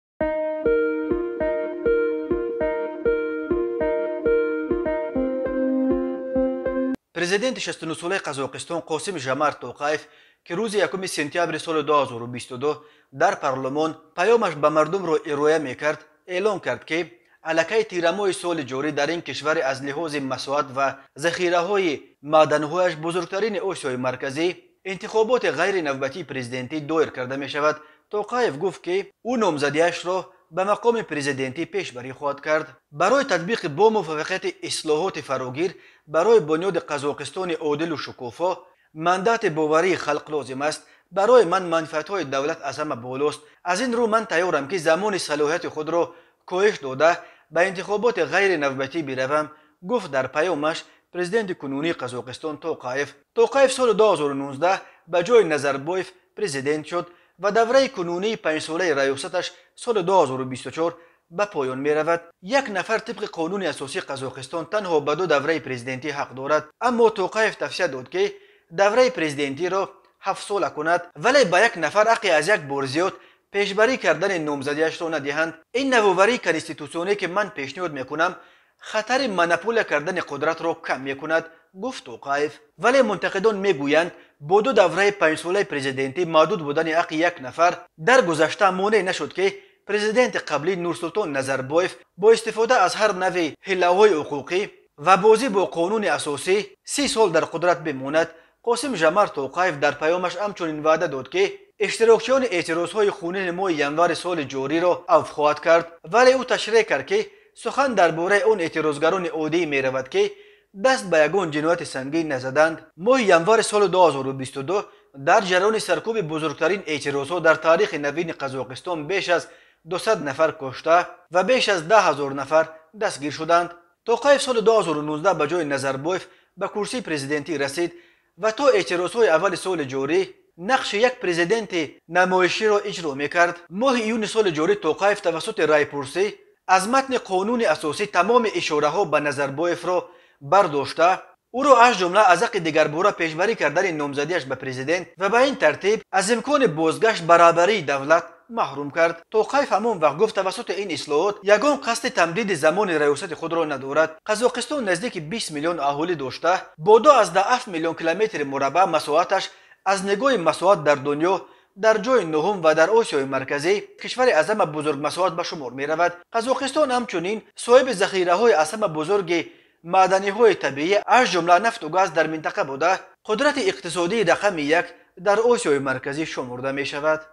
ЭЪЛОНИ ИНТИХОБОТИ ПЕШ АЗ МУҲЛАТИ ПРЕЗИДЕНТ ДАР ҚАЗОҚИСТОН
Президенти 69-солаи Қазоқистон Қосим-Жомарт Тоқаев, ки рӯзи 1 сентябри соли 2022 дар парлумон паёмаш ба мардумро ироа мекард, эълон кард, ки алакай тирамоҳи соли ҷорӣ дар ин кишвари аз лиҳози масоҳат ва захираҳои маъданҳояш бузургтарини Осиёи Марказӣ интихоботи ғайринавбатии президентӣ доир карда мешавад. Тоқаев гуфт, ки ӯ номзадиашро ба мақоми президентӣ пешбарӣ хоҳад кард.
 “Барои татбиқи бомуваффақияти ислоҳоти решаӣ ва фарогир барои бунёди Қазоқистони одилу шукуфо мандати боварии халқ лозим аст. Барои ман манфиатҳои давлат аз ҳама болост. Аз ин рӯ, ман тайёрам, ки замони салоҳиятҳои худро коҳиш дода, ба интихоботи ғайринавбатӣ биравам”, — гуфт дар паёмаш президенти кунунии Қазоқистон. Тоқаев соли 2019 ба ҷои Назарбоев президент шуд ва давраи кунунии панҷсолаи раёсаташ соли 2024 ба поён меравад. Як нафар, тибқи Қонуни асосии Қазоқистон, танҳо ба ду давраи президентӣ ҳақ дорад.
Аммо Тоқаев тавсия дод, ки давраи президентиро ҳафтсола кунанд, вале ба як нафар ҳаққи аз як бор зиёд пешбарӣ кардани номзадиашро надиҳанд. “Ин навоварии конститсионие, ки ман пешниҳод мекунам, хатари монополия кардани қудратро кам мекунад”,  — гуфт Тоқаев.
Вале мунтақидон мегӯянд, бо ду давраи панҷсолаи президентӣ маҳдуд будани ҳаққи як нафар дар гузашта монеъ нашуд, ки президенти қаблӣ Нурсултон Назарбоев бо истифода аз ҳар навъ ҳилаҳои ҳуқуқӣ ва бозӣ бо Қонуни асосӣ 30 сол дар қудрат бимонад.
Қосим-Жомарт Тоқаев дар паёмаш ҳамчунин ваъда дод, ки иштирокчиёни эътирозҳои хунин моҳи январи соли ҷориро афв хоҳад кард. Вале ӯ ташреҳ кард, ки сухан дар бораи он эътирозгарони оддие меравад, ки даст ба ягон ҷинояти сангин назадаанд.
 “Ин афв чеҳраҳои асосие, ки дар ташкили бетартибиҳо даст доштанд, ҳамчунин нафарони айбдор ба кӯшиши ғасби ҳукуматро фаро нахоҳад гирифт”, — изҳор дошт Тоқаев дар паёмаш.
Моҳи январи соли 2022 дар ҷараёни саркӯби бузургтарин эътирозҳо дар таърихи навини Қазоқистон беш аз 200 нафар кушта ва беш аз 10 ҳазор нафар дастгир шуданд.